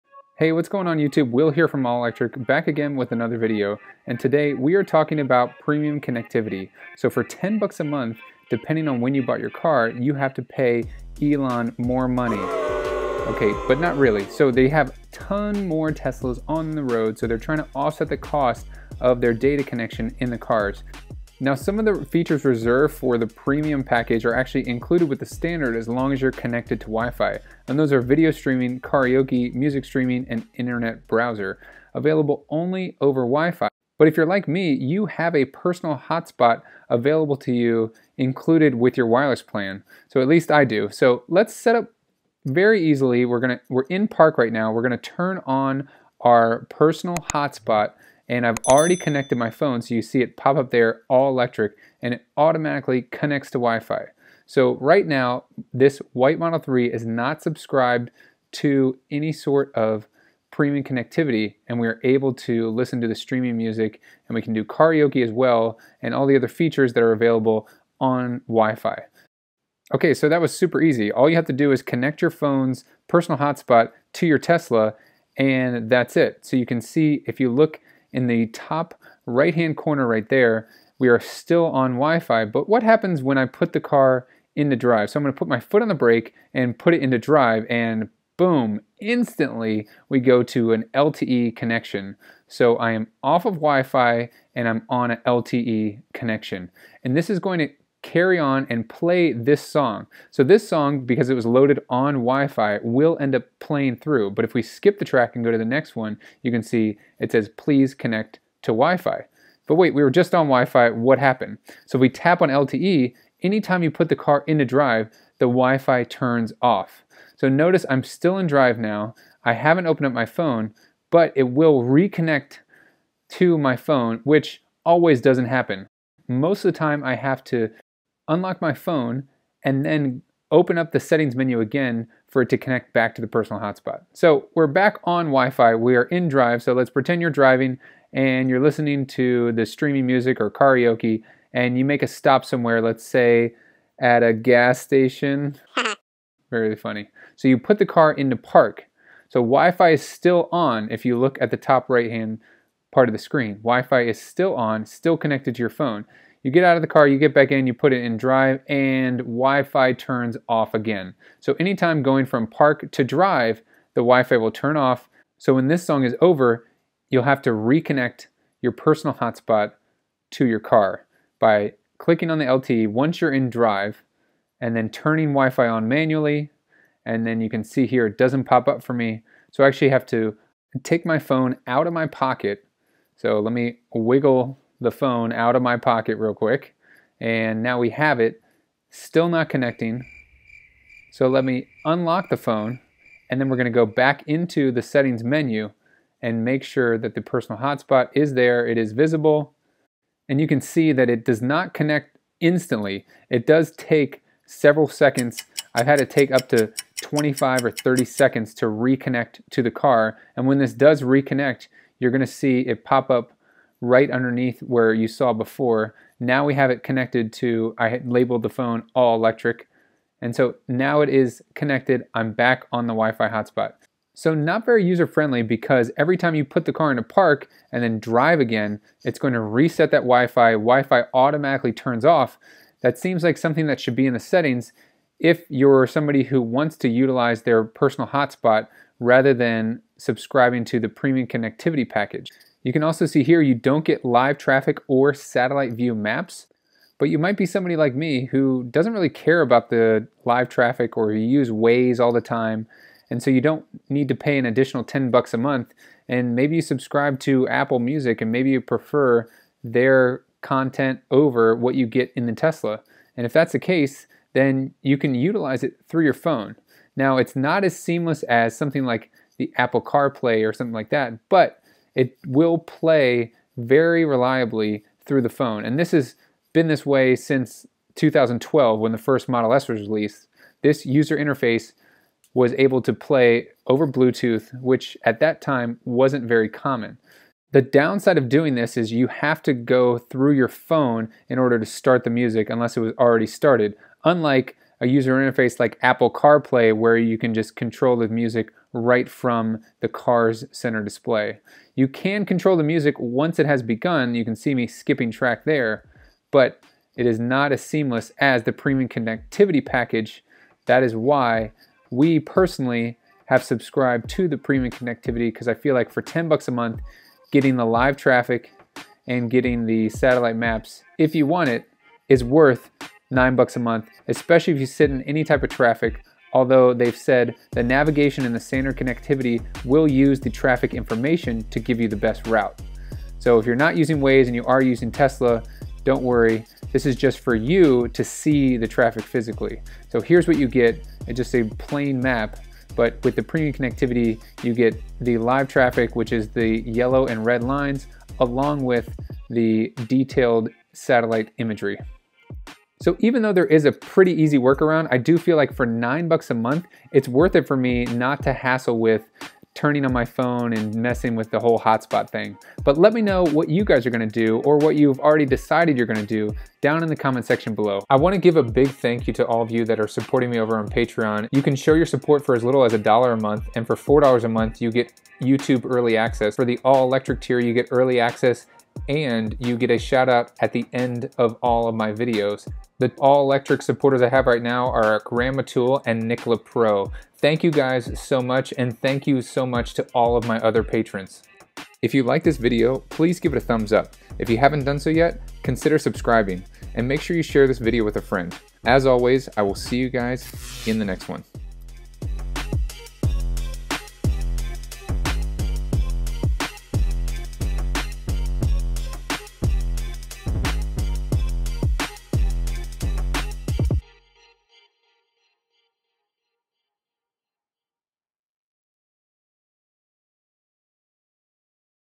Tesla How To | Free* Premium Connectivity | Simple Trick Gets You Almost All the Features!
Every new Tesla comes with one free month. After that month is up, you'll need to decide if you want to pay an extra $9.99 monthly for the added features.

Get 1,000 miles of FREE Supercharging when you purchase or lease a Tesla!!!